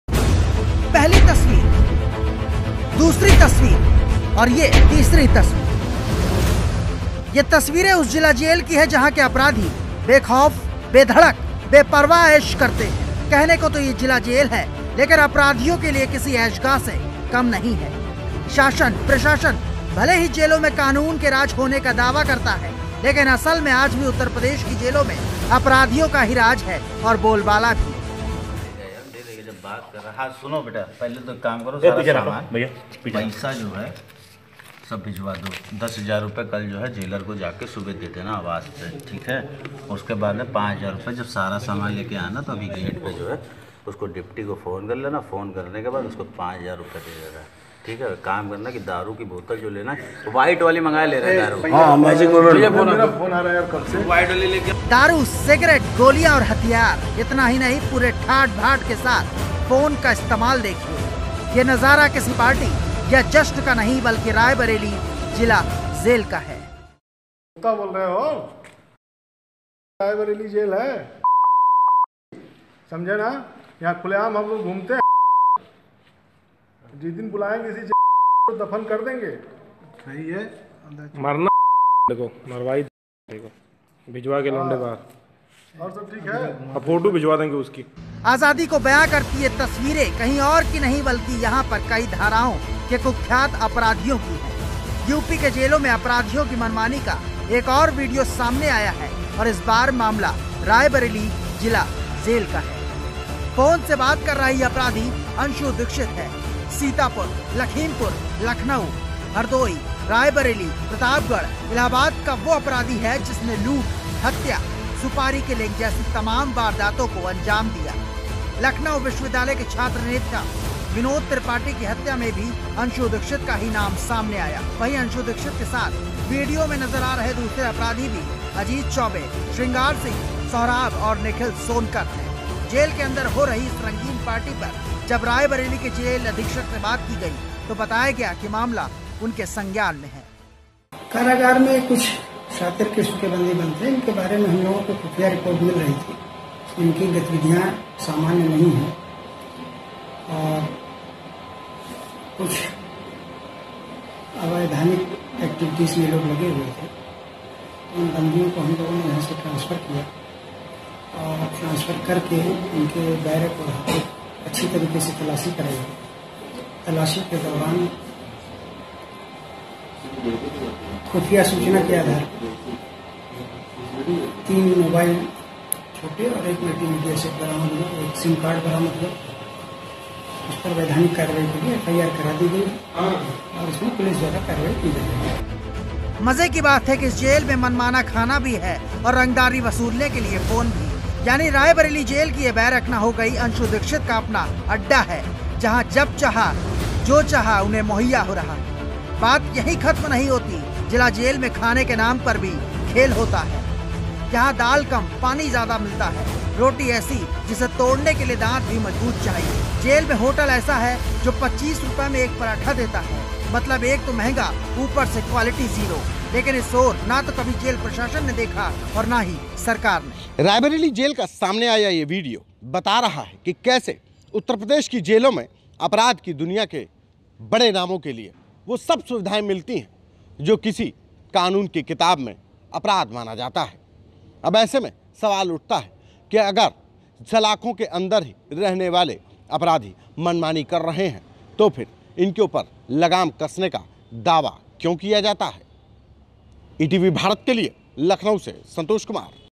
रायबरेली मामला : Etv की खबर का असर, जेल अधीक्षक समेत 6 सस्पेंड
लखनऊ : रायबरेली जेल में शातिर गैंगस्टर अंशु दीक्षित के वायरल वीडियो के मामले में बड़ा मोड़ आया है. इस मामले में जेल अधीक्षक समेत 6 को सस्पेंड किया गया है. दरअसल, वायरल वीडियो में खुलासा हुआ था कि अपराधियों को जेल में वीआईपी जैसा ट्रीटमेंट दिया जा रहा था.